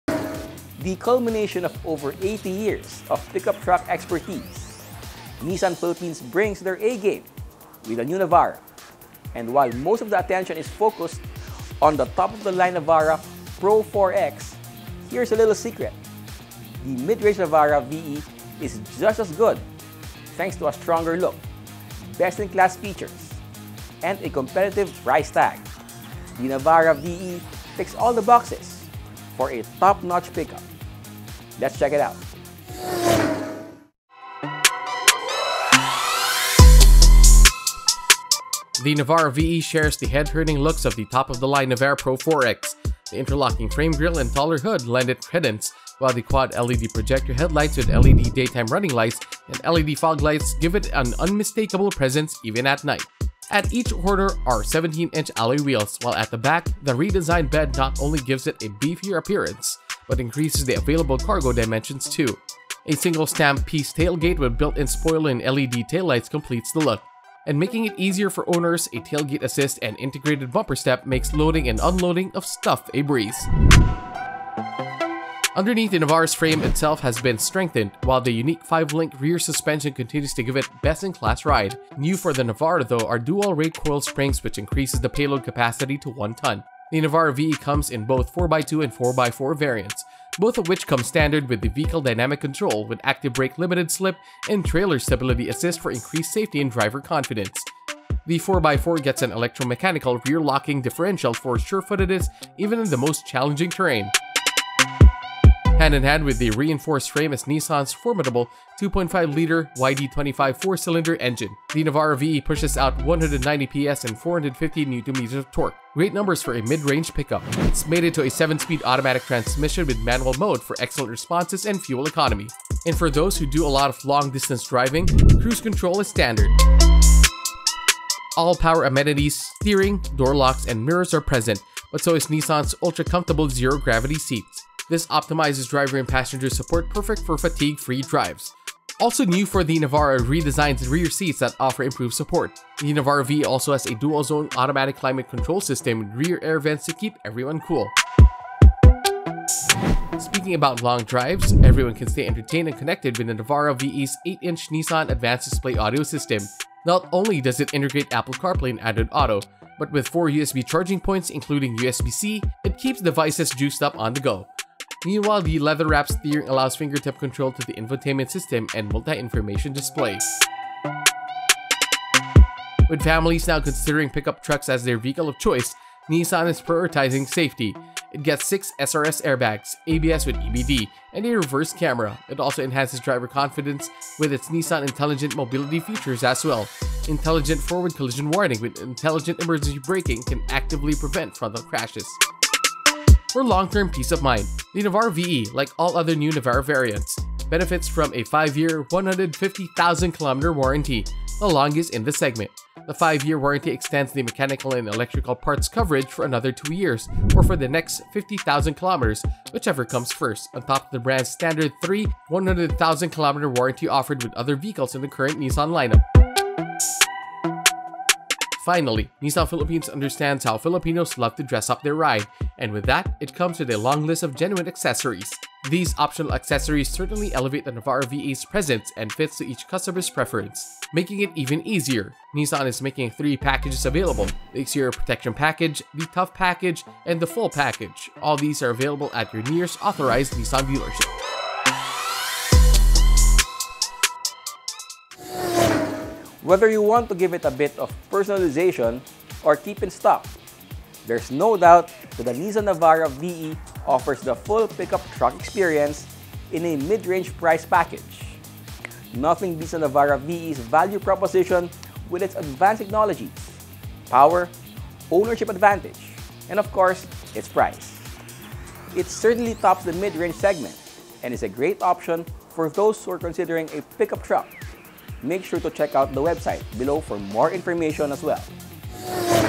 The Nissan Navara VE Ticks All The Boxes For A Top-Notch Pickup Truck | Special Feature
Let’s check out how the mid-range New Nissan Navara VE ticks all the boxes for a top-notch pickup!